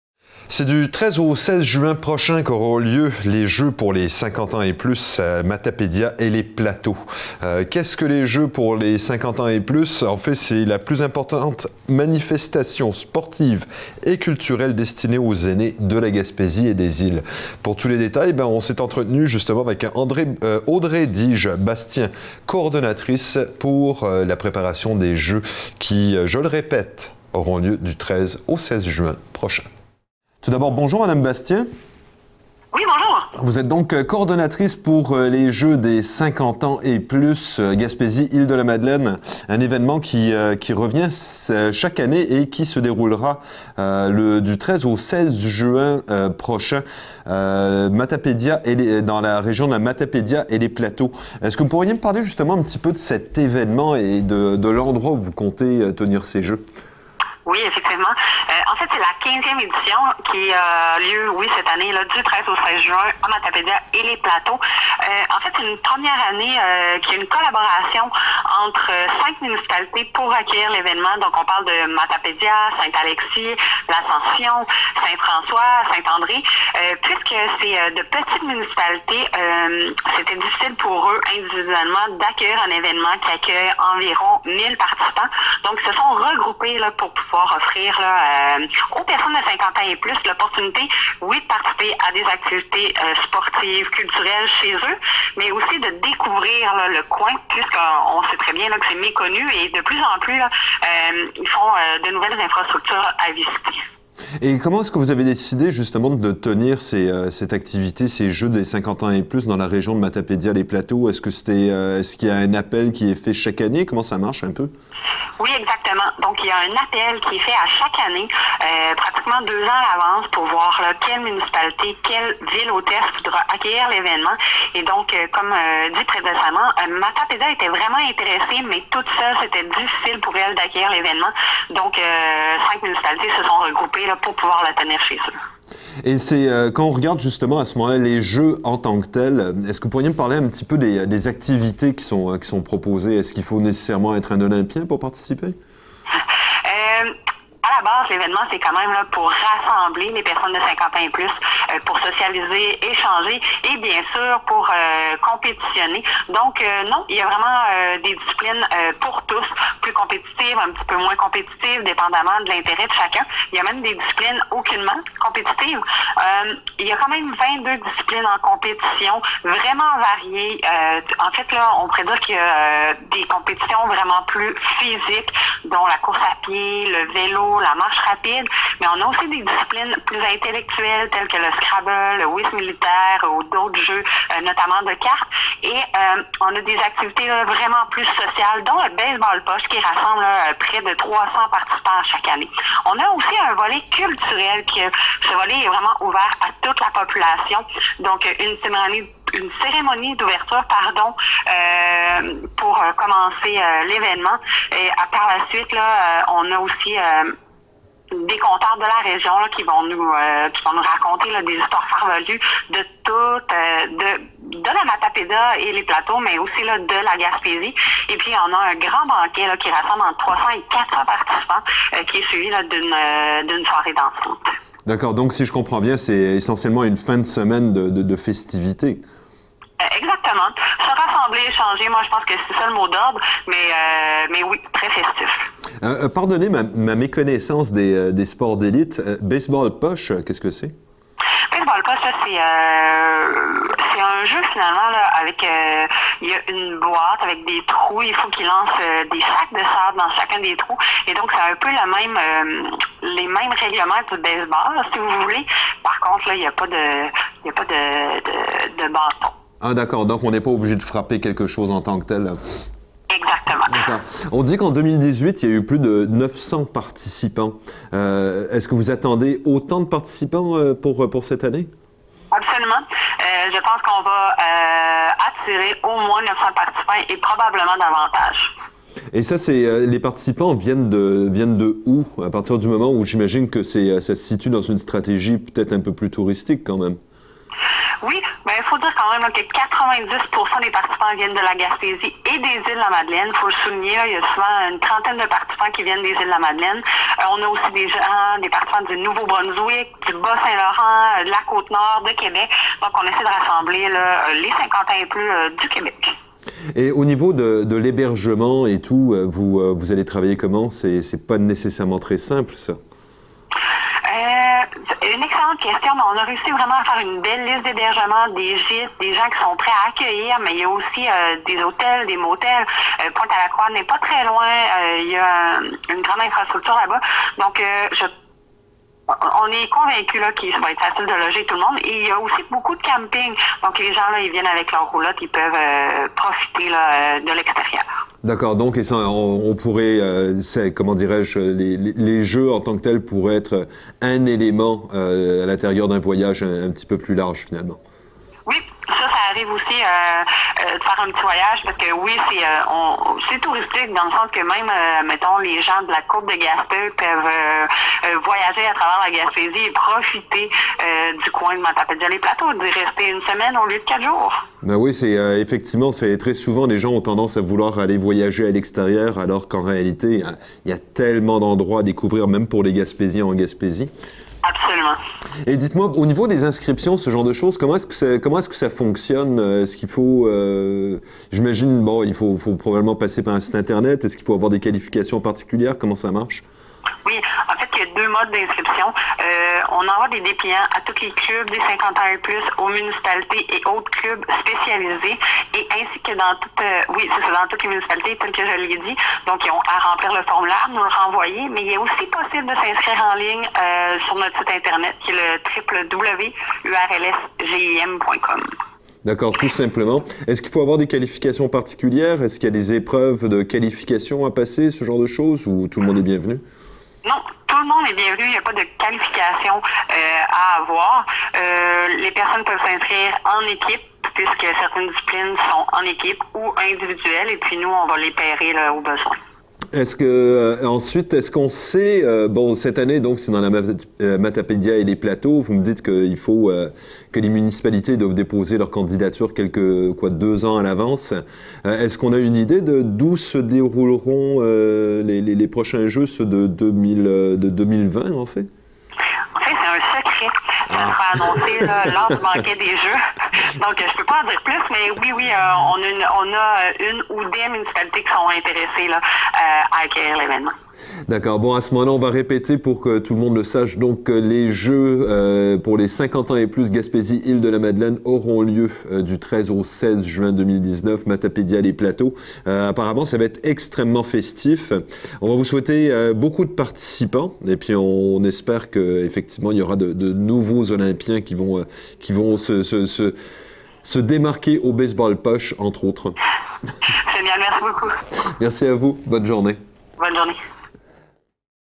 Jeux des 50 ans et plus : pas juste pour les sportifs
Du 13 au 16 juin, la région de Matapédia-et-les-Plateaux recevra des centaines d’aînés désirant se mesurer dans diverses activités sportives, ludiques ou intellectuelles. Audrey Bastien, coordonnatrice des jeux, nous parle de la plus importante manifestation sportive et culturelle destinée aux aînés de la Gaspésie et des Îles-de-la-Madeleine.